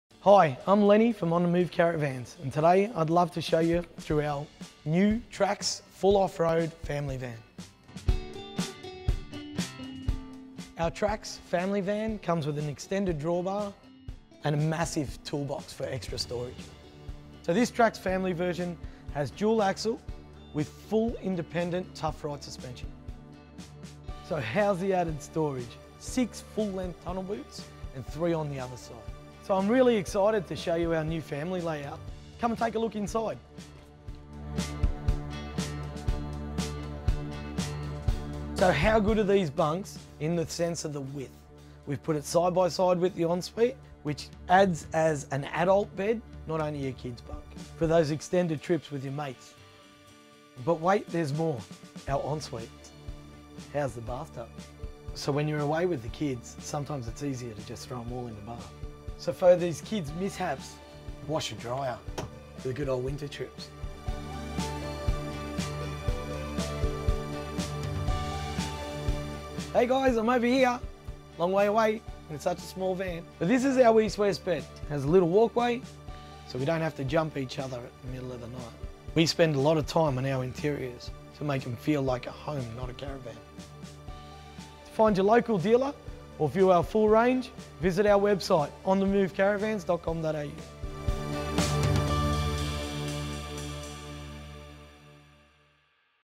On The Move Traxx SII Family Off Road - Wide Bunks!
Independent suspension, 6″ chassis, solar power, dual batteries and loads more features – too much to mention – this van will be your home away from home with ease.

Stepping into this van, I can assure you, you will feel right at home with the space and design practicality this van offers. No matter the size, you will not be disappointed.